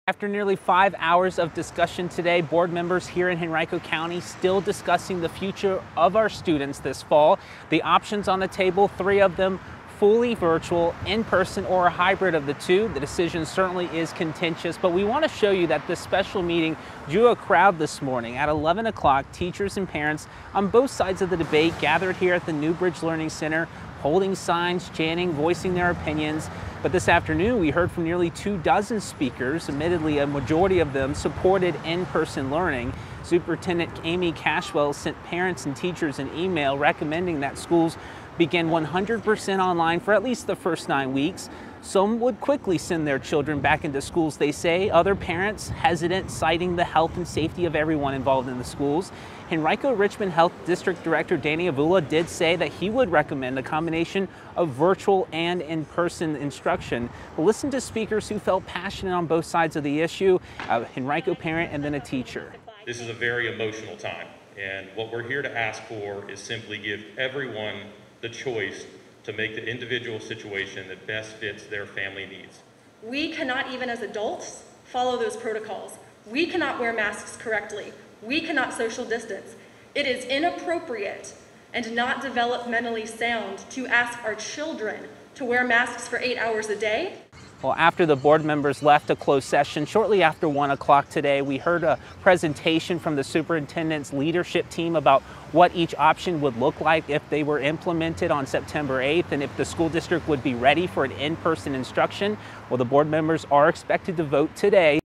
Online or in class? Henrico votes on how to start the school year
The Henrico County School Board will vote Thursday on the county's school reopening plan.